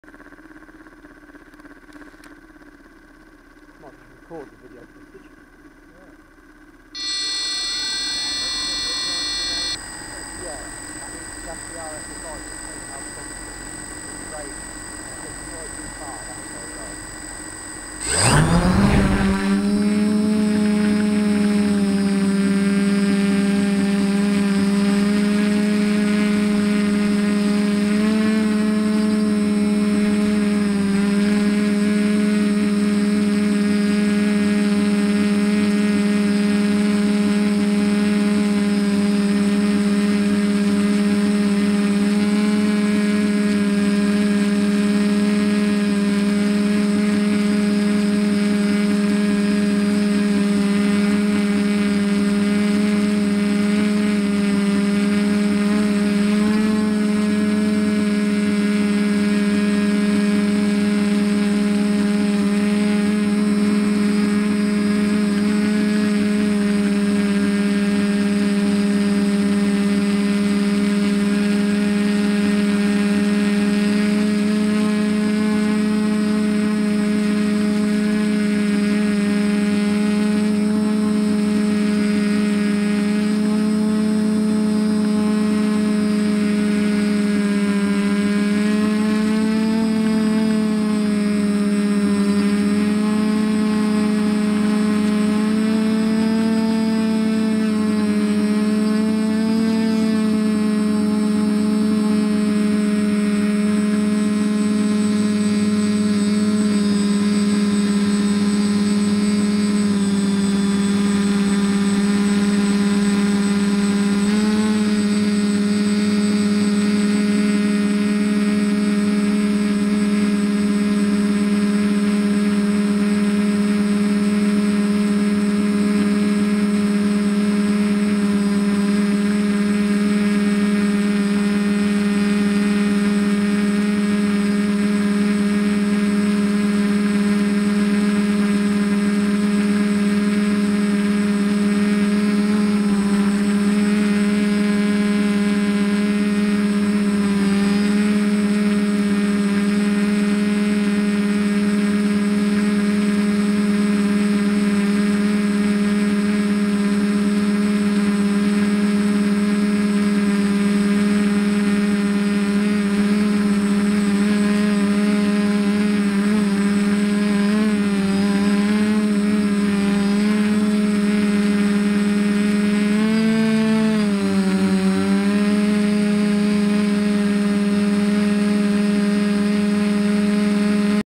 TBS Discovery with Pixhawk flight controller - first flight
TBS Discovery
Pixhawk
Turnigy 30Amp ESC
Sunnysky motors
RC timer carbon props 10x5
Ghetto Gimbal
SJ4000 camera